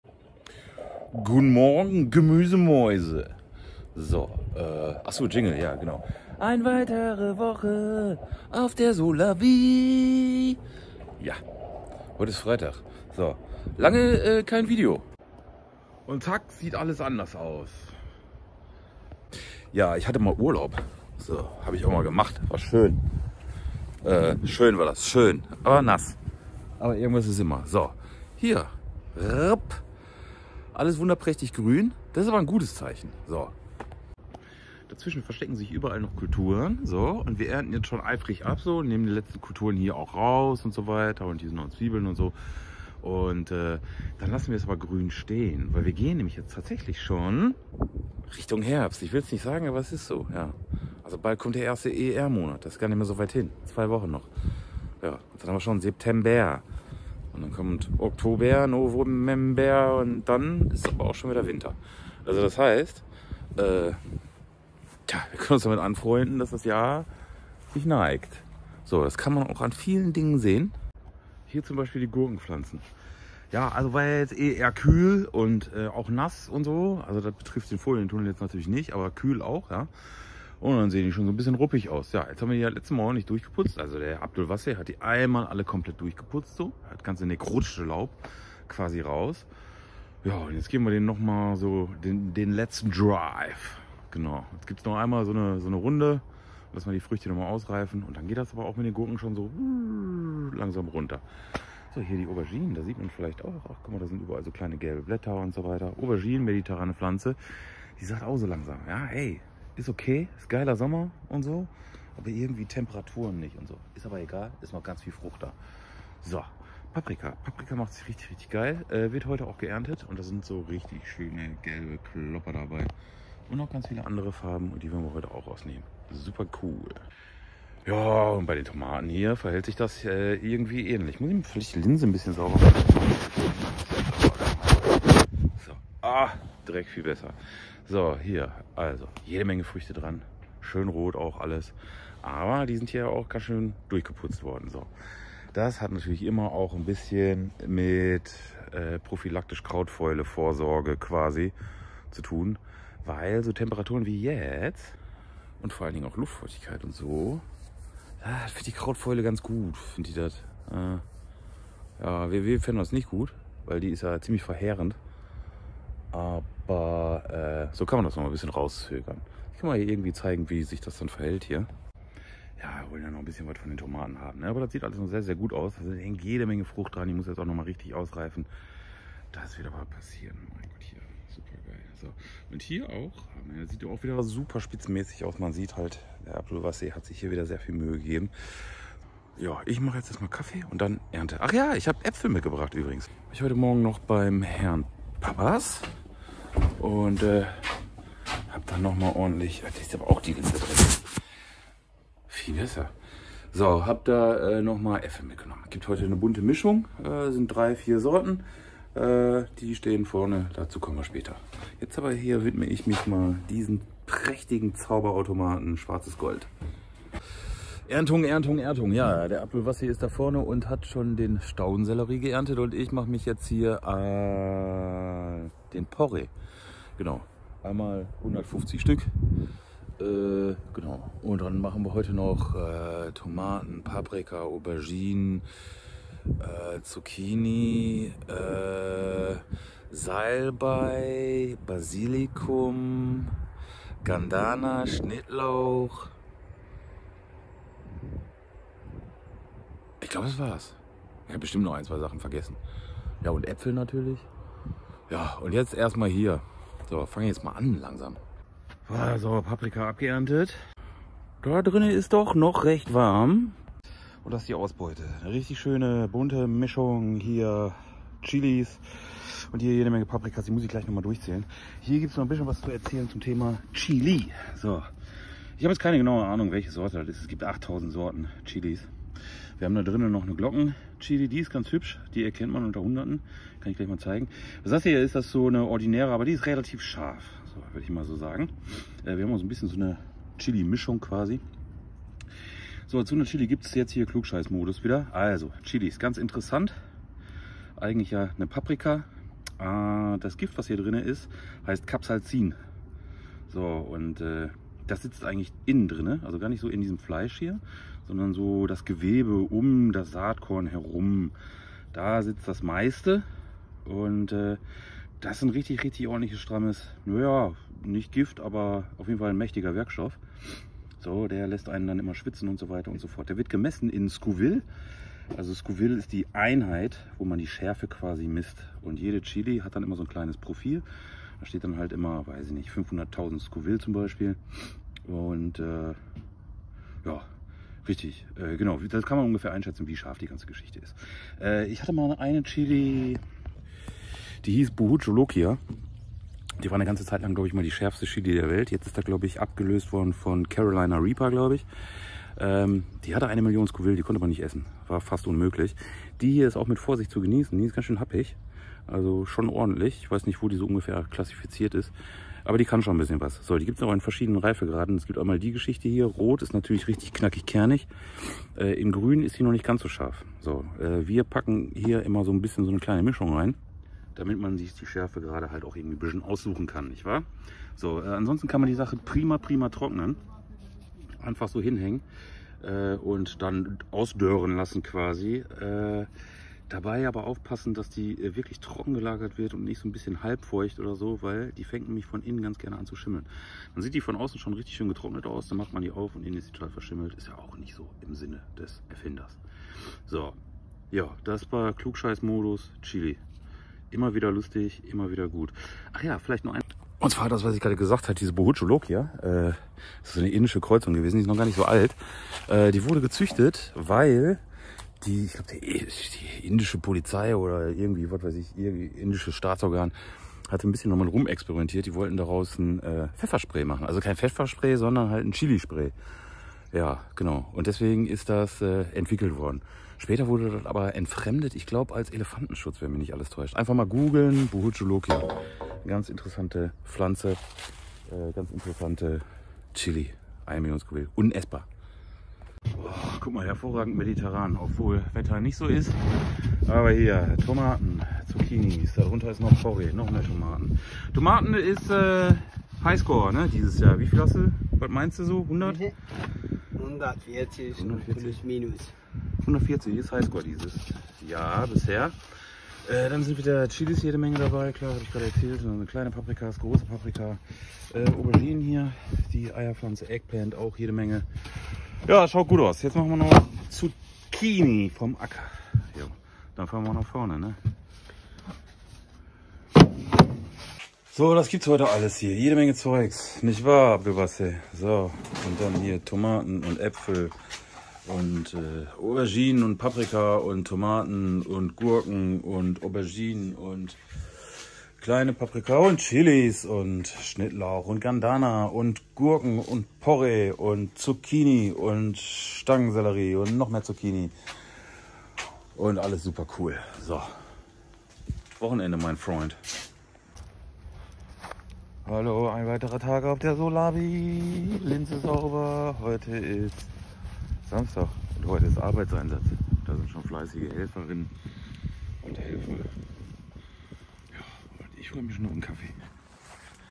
Solawi Niederrhein Folge 21 / 2023 (KW31) Der Herbst ruft.
Solidarischer Gemüseanbau ohne chemische Hilfsmittel.
Unverdorben, lecker, gesund und nachhaltig.
Mit und nicht gegen die Natur arbeiten.

Folgt uns ein ganze Saison. Solawi Niederrhein mit den Gärtnern Abdul und Ben.